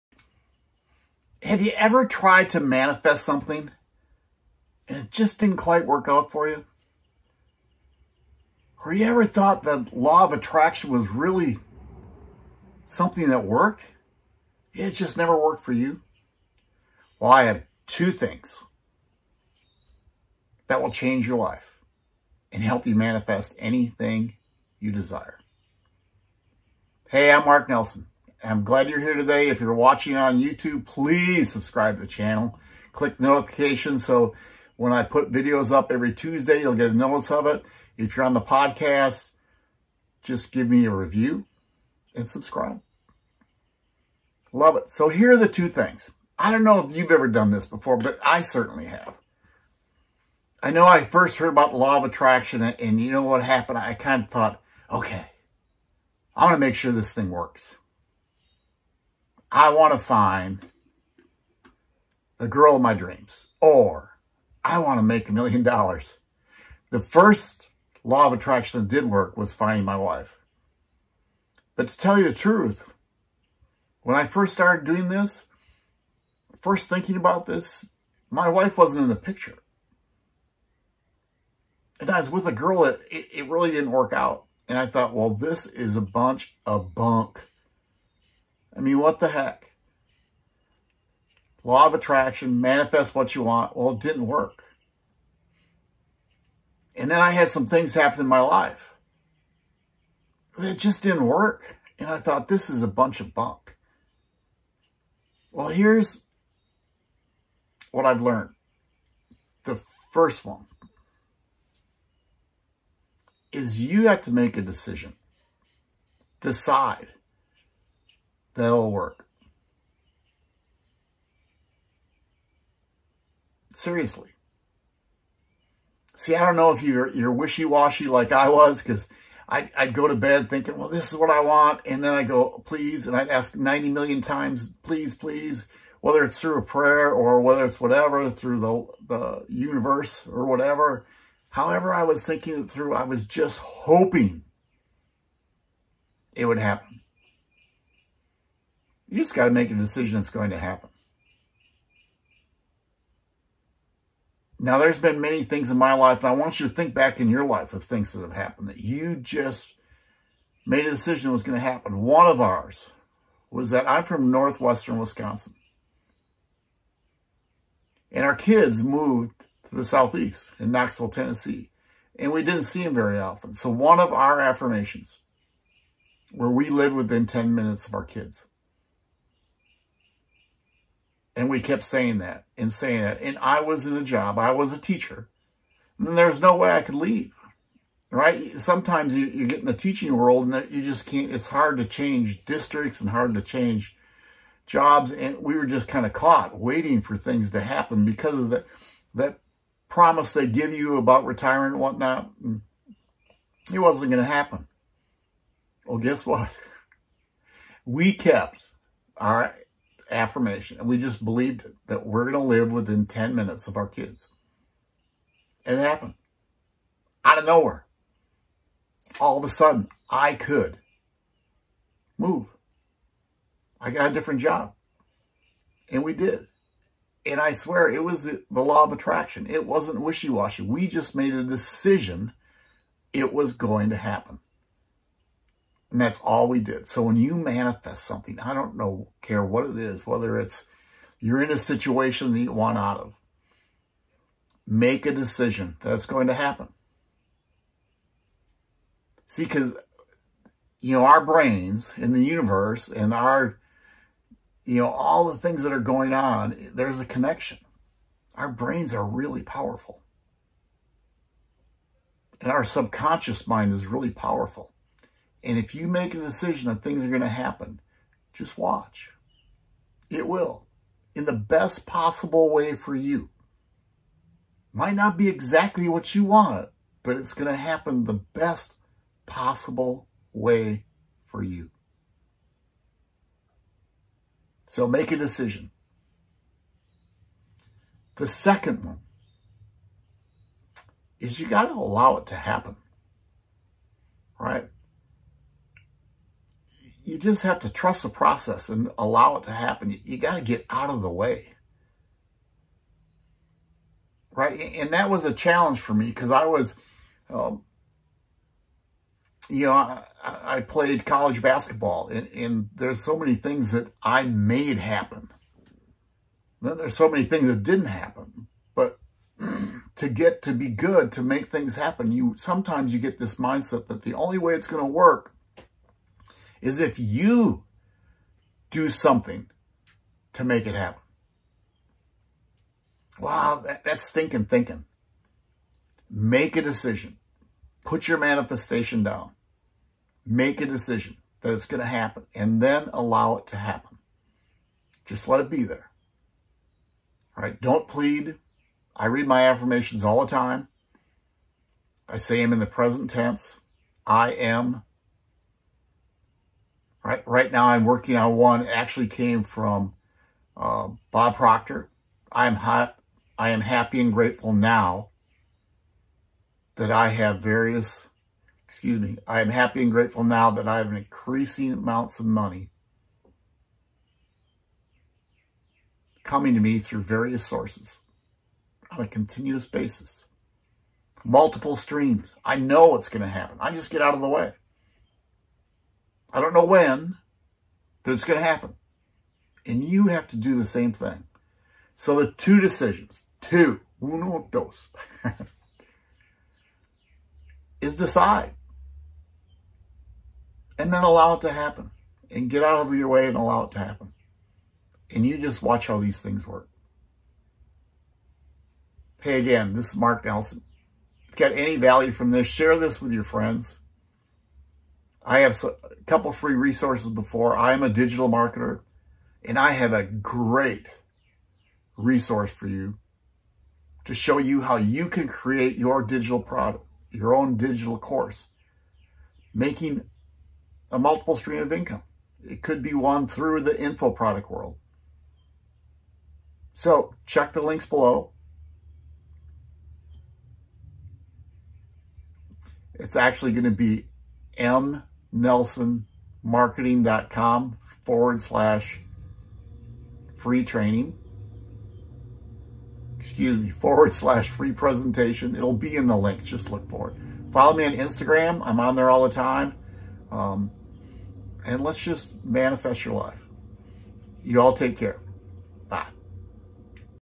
2 Things To Manifest Your Dreams
2 Things To Manifest Your Dreams

Have you ever wondered why some people seem to achieve all their dreams.

I know I did for awhile.

Here are two things you need to do.

Make a decision

Actually decide what you want to manifest

Then allow it to happen.

Free Training On How To Create An Online Course